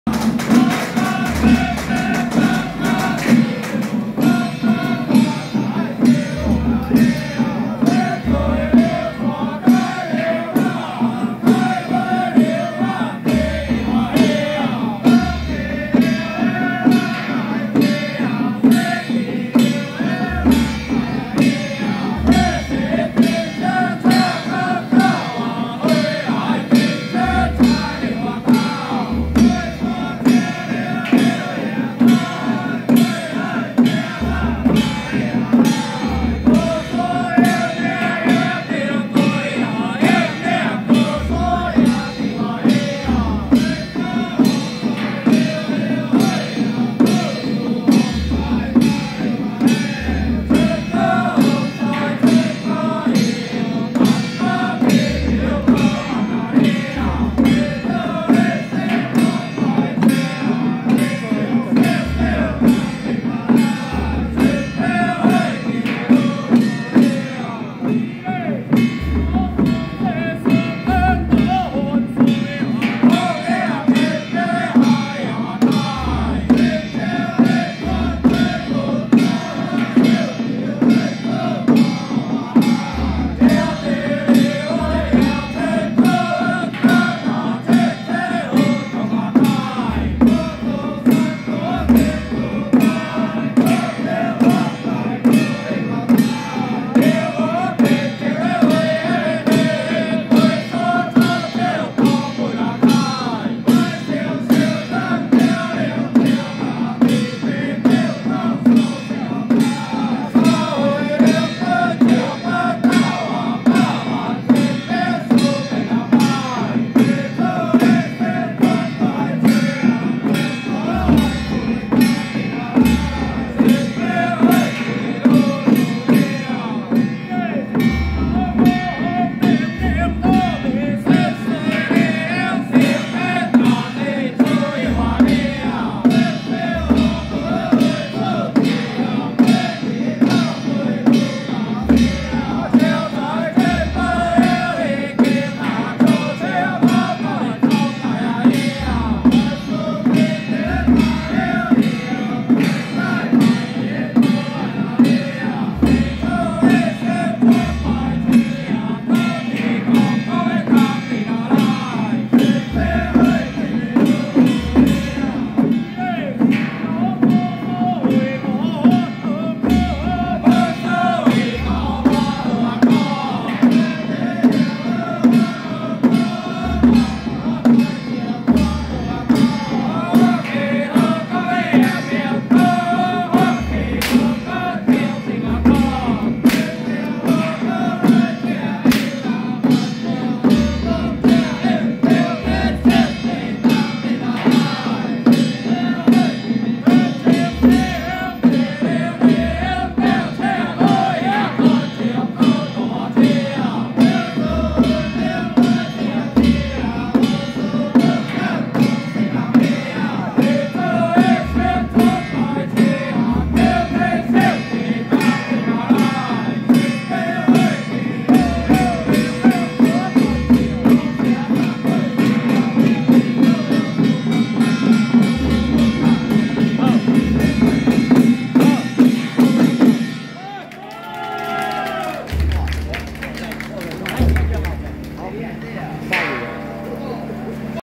【台中太平受天宮+半線閭心壇 聯合小法團】~111台中聖帝堂九天大聖帝往鶯歌宏德宮孫臏廟謁祖進香
台中太平受天宮+半線閭心壇 聯合小法團